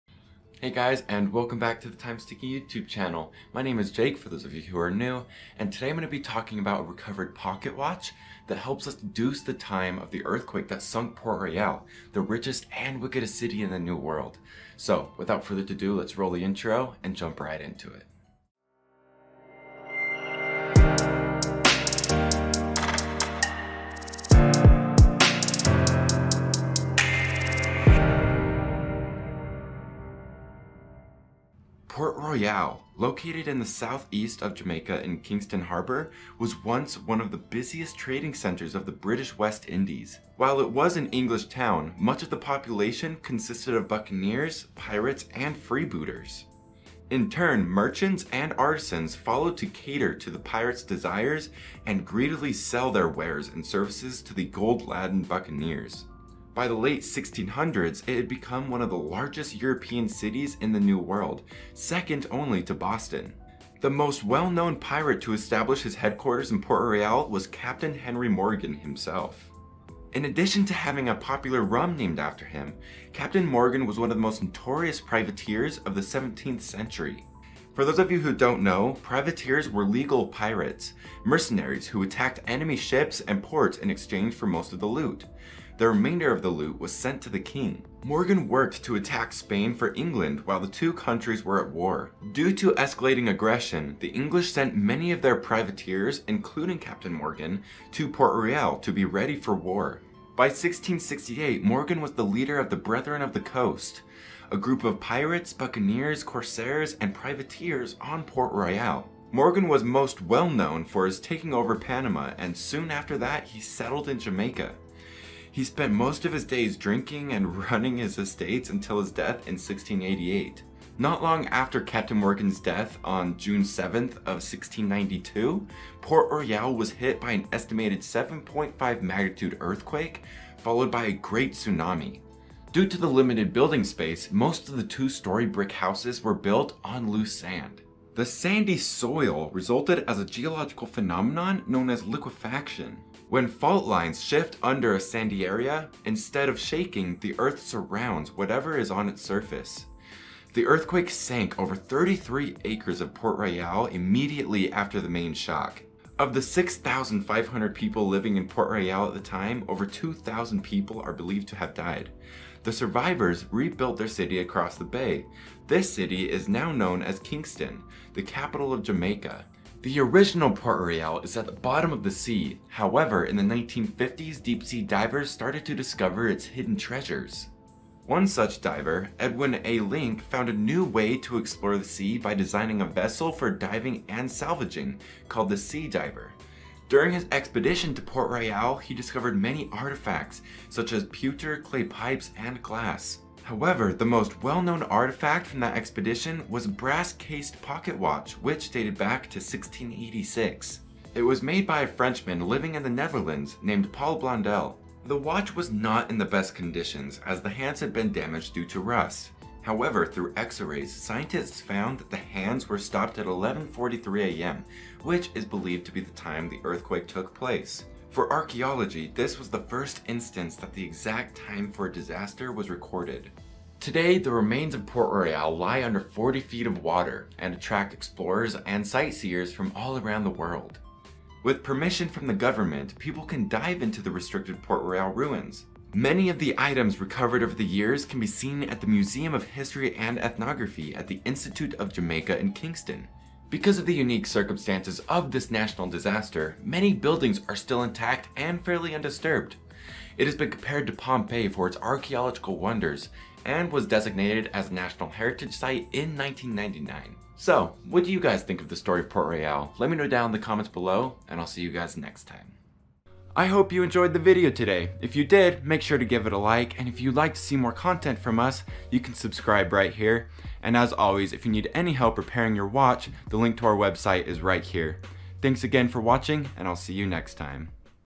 This Watch Told Us The EXACT Time The City of Pirates Sunk Into The Ocean
Timepieces and pirates may not have a clear connection, but thanks to a recovered pocket watch, we can deduce the time of the earthquake that sunk Port Royal, the richest and wickedest city in the new world.